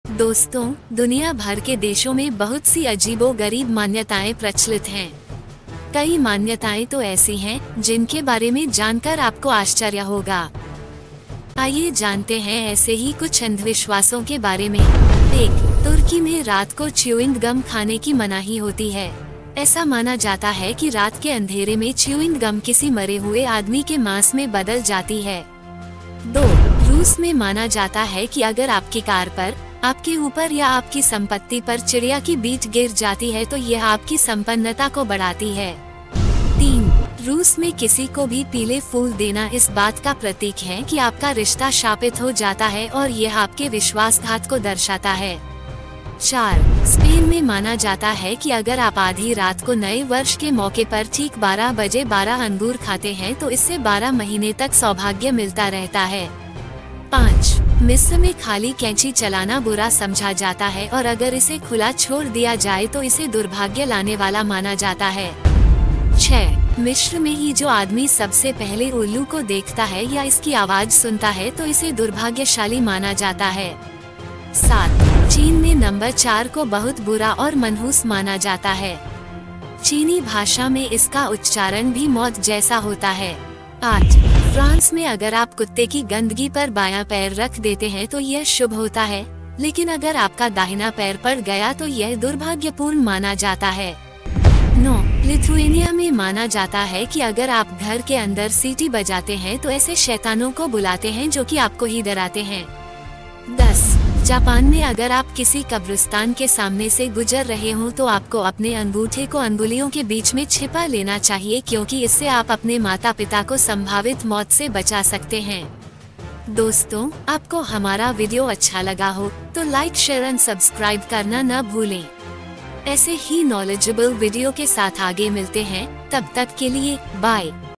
दुनिया कि 10 अजीबो गरीब मान्यताएं।🔥
आइए जानते हैं ऐसे ही कुछ अंधविश्वासों के बारे में...